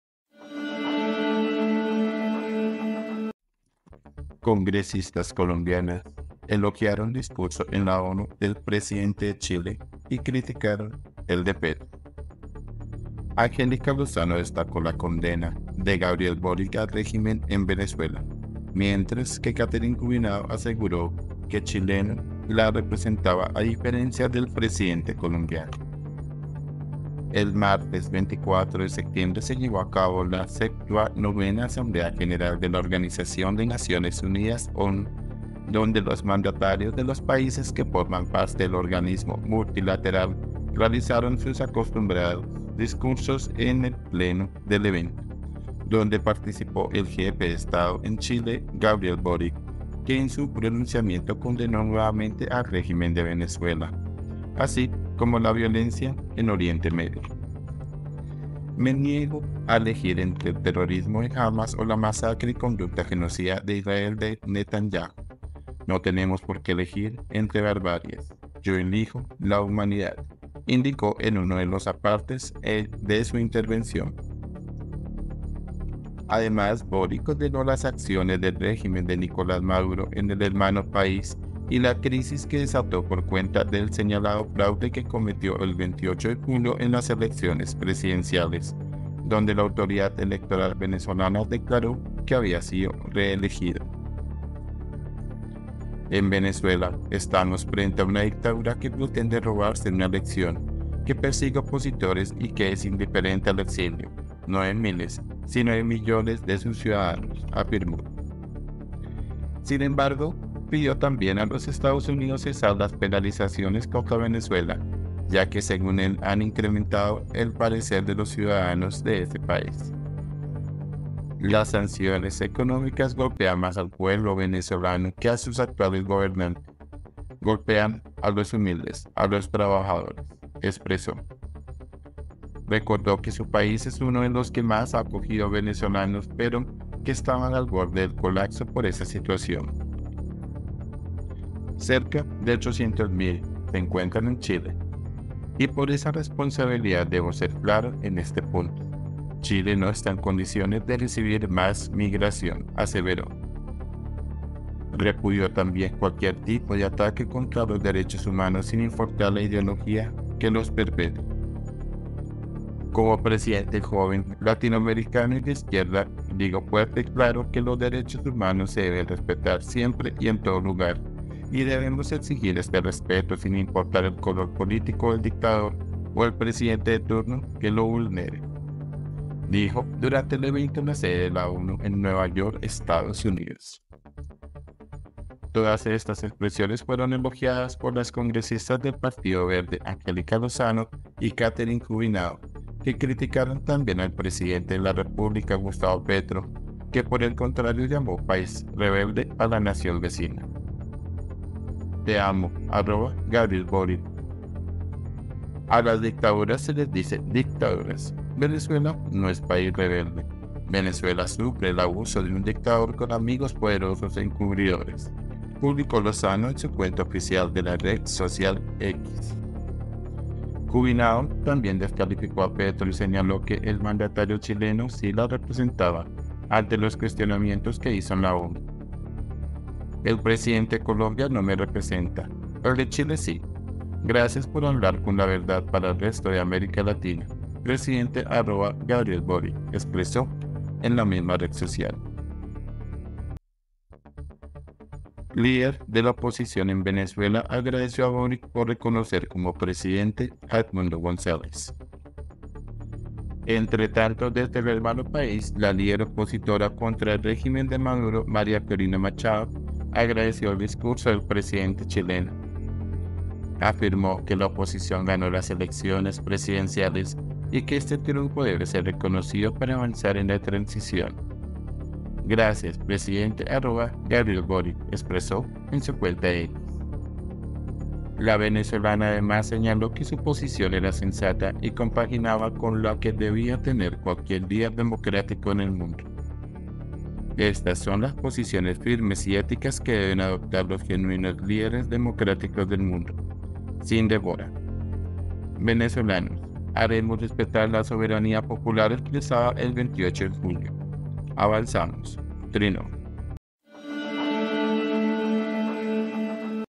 🌐🚨 ELOGIOS A DISCURSO DE BORIC Y DURAS CRÍTICAS AL DE PETRO EN LA ONU
Congresistas colombianas elogiaron discurso en la ONU del presidente de Chile y criticaron el de Petro
Angélica Lozano destacó la condena de Gabriel Boric al régimen en Venezuela, mientras que Catherine Juvinao aseguró que chileno “la representaba” a diferencia del presidente colombiano
INFOBAE